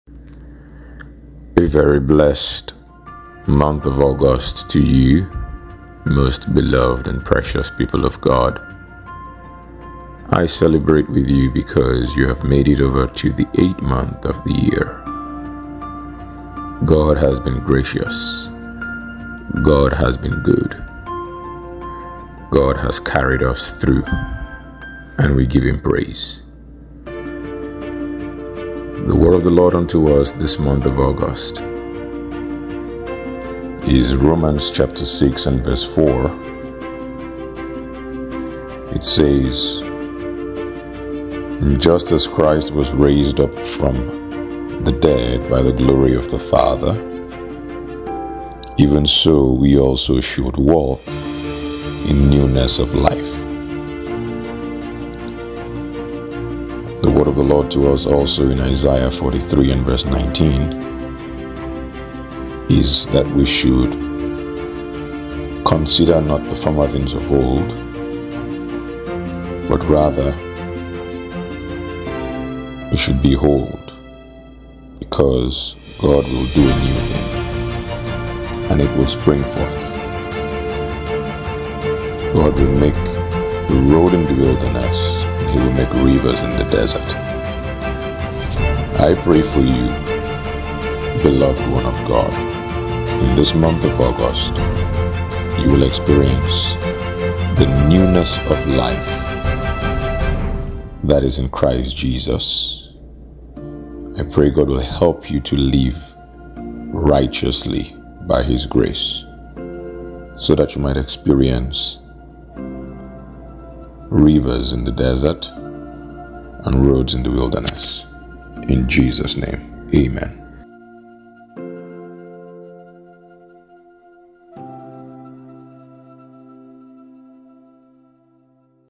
Powerful Declaration over August 2023 by PBC Lead Steward
Happy New Month PBC Family!

By faith, believe and receive the Powerful Declarations over August 2023 by PBC Lead Steward; Dammy Olu-Ayeni.